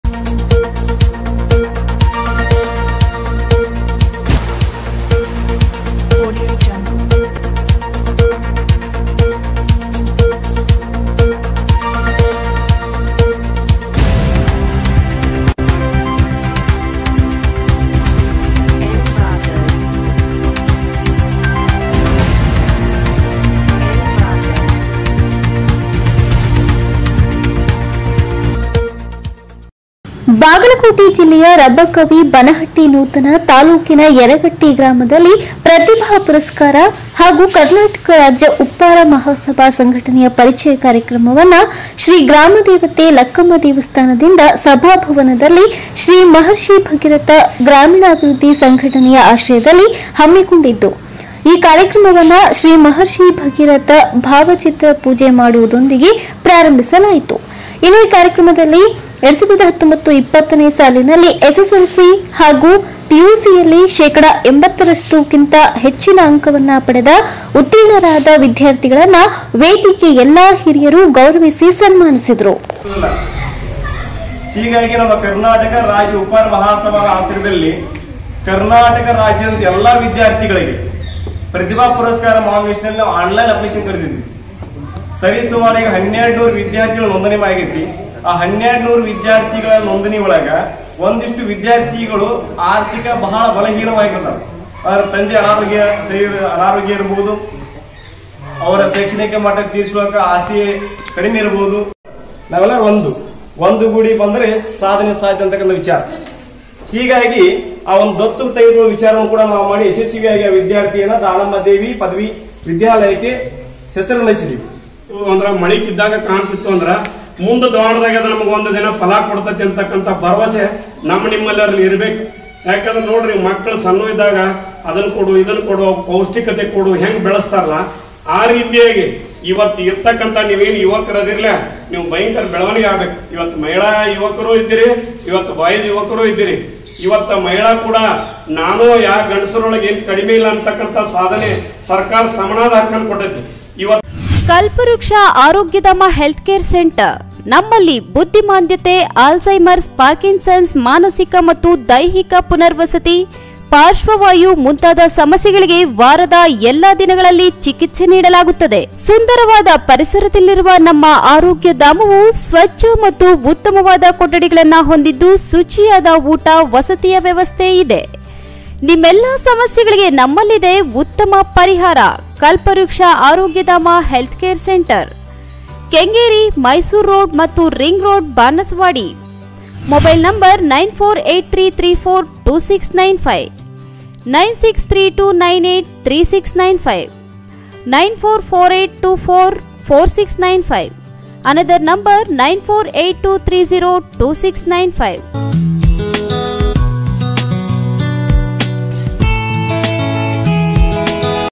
ಮಾಹಿತಿ ನಿಡದ ಕಾರಣ ಪಂಚಾಯತಿ ಮುಂದೆ ದರಣಿ sachi tv
ಬಾಗಲಕೋಟೆ ಜಿಲ್ಲೆಯ ರಬಕವಿ-ಬನಹಟ್ಟಿ ನೂತನ ತಾಲ್ಲೂಕಿನ ಯರಗಟ್ಟಿ ಗ್ರಾಮದಲ್ಲಿ ಪ್ರತಿಭಾ ಪುರಸ್ಕಾರ ಹಾಗೂ KRUMS (ಕರ್ನಾಟಕ ರಾಜ್ಯ ಉಪ್ಪಾರ ಮಹಾಸಭಾ) ಸಂಘಟನೆಯ ಪರಿಚಯ ಕಾಯ೯ಕ್ರಮವನ್ನು ಶ್ರೀ ಗ್ರಾಮದೇವತೆ ಲಕ್ಕಮ್ಮ ದೇವಸ್ಥಾನದ ಸಭಾಭವನದಲ್ಲಿ ಶ್ರೀ ಮಹಷಿ೯ ಭಗೀರಥ ಗ್ರಾಮೀಣಾಭಿವೃದ್ಧಿ ಸಂಘಟನೆಯ ಆಶ್ರಯದಲ್ಲಿ  ಹಮ್ಮಿಕೊಂಡಿದ್ದು ಈ ಕಾಯ೯ಕ್ರಮವನ್ನು ಶ್ರೀ ಮಹಷಿ೯ ಭಗೀರಥರ ಭಾವ ಚಿತ್ರ ಪೂಜೆ ಮಾಡುವುದರೊಂದಿಗೆ ಪ್ರಾರಂಭಿಸಲಾಯಿತು.

ಈ ಕಾಯ೯ಕ್ರಮದಲ್ಲಿ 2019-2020ನೇ ಸಾಲಿನಲ್ಲಿ ಎಸ್.ಎಸ್.ಎಲ್.ಸಿ  ಹಾಗೂ ದ್ವಿತೀಯ ಪಿ.ಯೂ.ಸಿ ಯಲ್ಲಿ ಶೇಖಡಯ 80% ಕಿಂತ ಹೆಚ್ಚಿನ ಅಂಕ ಪಡೆದು ಉತ್ತಿಣ೯ರಾದ ವಿದ್ಯಾರ್ಥಿಗಳನ್ನು ವೇದಿಕೆ ಎಲ್ಲಾ    ಹಿರಿಯರು ಗೌರವಿಸಿ ಸನ್ಮಾಸಿದ್ದಾರು.

ಈ ಕಾಯ೯ಕ್ರಮದ ಅಧ್ಯಕ್ಷತೆಯನ್ನು ವಹಿಸಿದ ಶ್ರೀ ಪುಂಡಲೀಕ ಗೋಪಾಳಿಯವರು ಪ್ರತಿ ವರ್ಷ ಈ ಪ್ರತಿಭಾ ಪುರಸ್ಕಾರ ಕಾಯ೯ಕ್ರಮವನ್ನು  ಮುಂದುವರಿಸಿಕೊಂಡು ಹೋಗುವದಾಗಿ ತಿಳಿಸಿದರು ಹಾಗೂ ಕರ್ನಾಟಕ ರಾಜ್ಯ ಉಪ್ಪಾರ ಮಹಾಸಭಾ ಕಾಯ೯ಕ್ಕೆ ಮೆಚ್ಚುಗೆ ವ್ಯಕ್ತಪಡಿಸಿದರು. ಬಾಗಲಕೋಟೆ ಜಿಲ್ಲೆಯ ಸಮಾಜದ ಅಧ್ಯಕ್ಷರಾದ ಶ್ರೀ ಭೀಮಶಿ ಪಾಟೀಲ ಅವರು ಕಾಯ೯ಕ್ರಮವನ್ನು ಉದ್ದೇಶಿಸಿ ಮಾತನಾಡಿದರು. ಕನಾ೯ಟಕ ರಾಜ್ಯ ಉಪ್ಪಾರ ಮಹಾಸಭಾ (ರಿ) ಬಾಗಲಕೋಟೆ ಜಿಲ್ಲೆಯ ಸಂಚಾಲಕರಾದ ರಾಘವೇಂದ್ರ ನೀಲನ್ನವರ ಅವರು  ಸಂಘಟನೆಯು ಹಮ್ಮಿಕೊಂಡಿರುವ ವಿವಿಧ ಉದ್ದೇಶಗಳ ಮಾಹಿತಿಯನ್ನು  ಸವಿಸ್ತಾರವಾಗಿ ತಿಳಿಸಿಕೋಟ್ಟರು. ಉಪ್ಪಾರರು ಶ್ರೀ ರಾಮನ ವಾರಸುದಾರರು ಎಂದು ಹೇಳುವುದರ ಮೂಲಕ ಎಲ್ಲಾ ವಿದ್ಯಾರ್ಥಿಗಳನ್ನು ಉದ್ದೇಶಿಸಿ ಅವರ ಮುಂದಿನ ಶಿಕ್ಷಣದ ಮಾಗ೯ಗಳನ್ನು ತಿಳಿಸಿಕೋಟ್ಟರು.